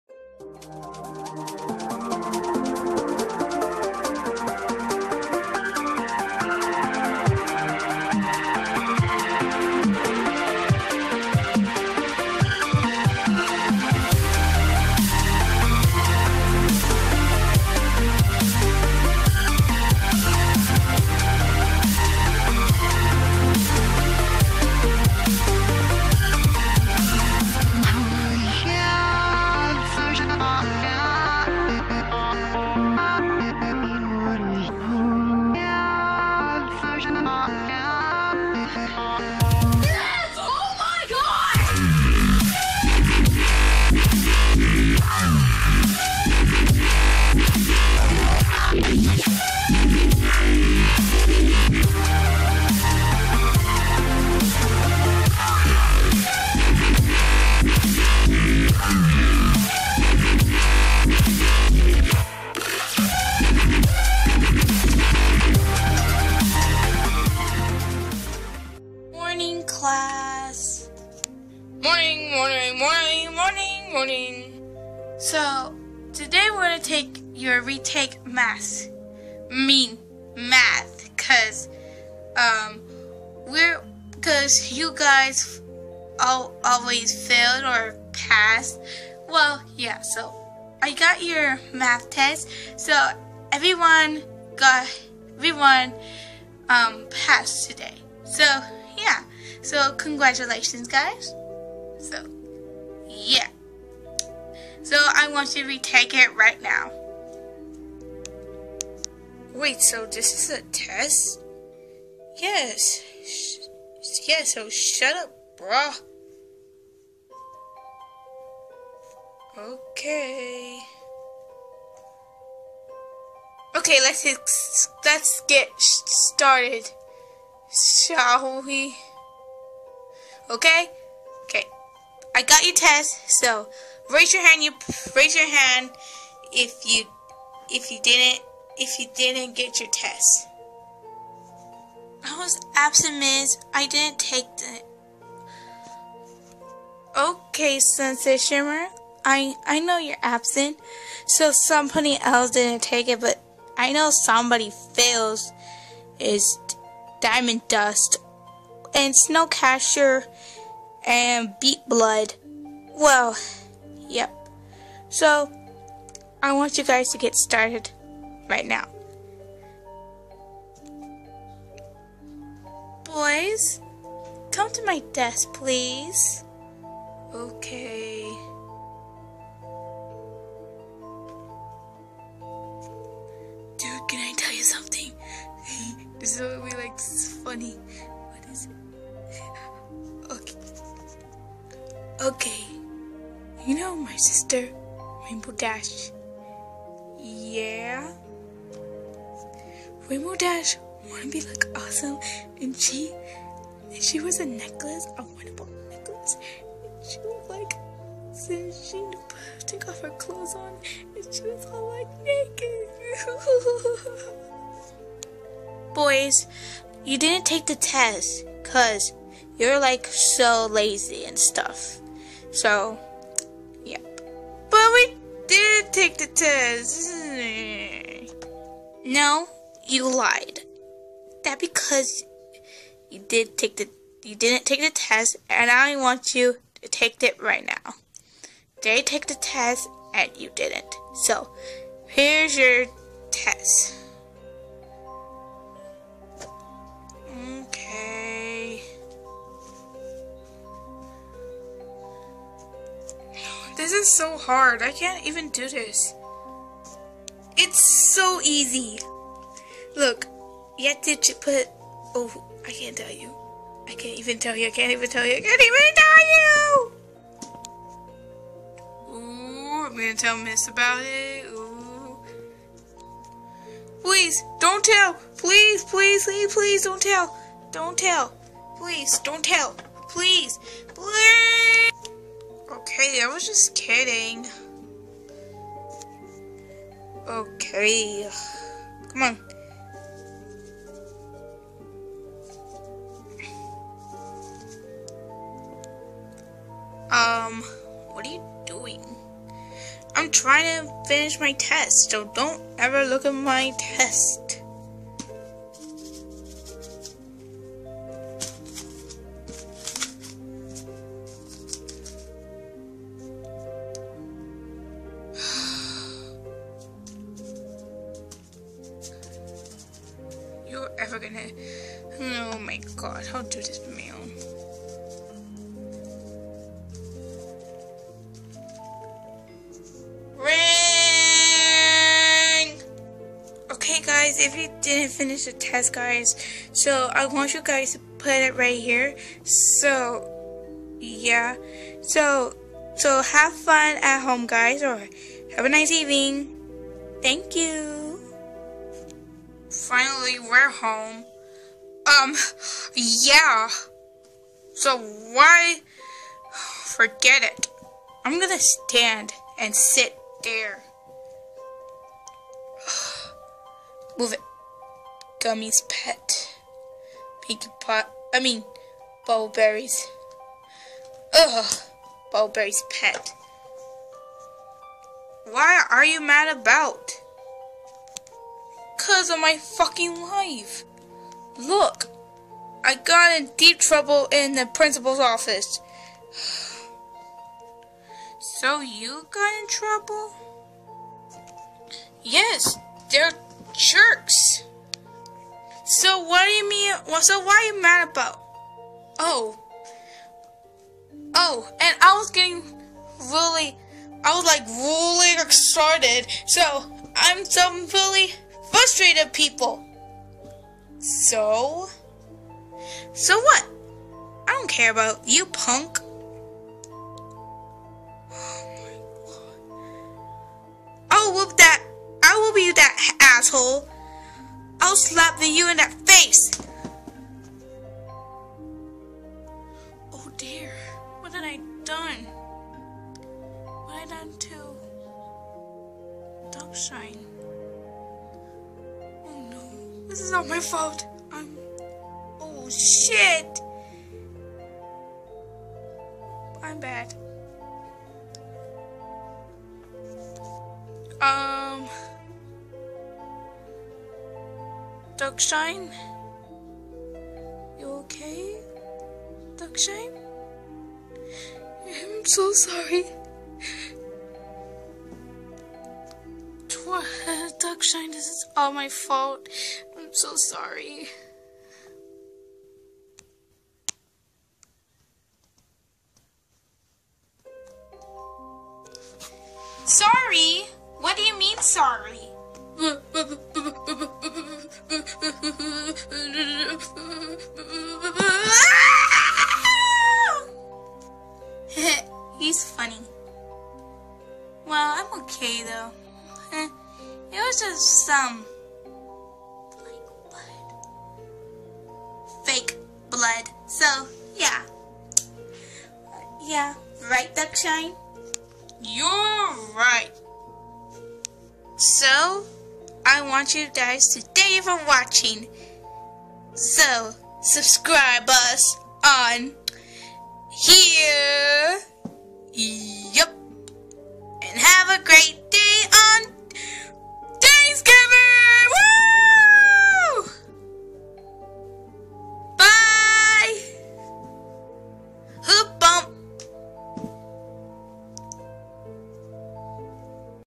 SLAP MINI MOVIE
Have you seen the video when dusk shine and Rainbow blitz seen to slap?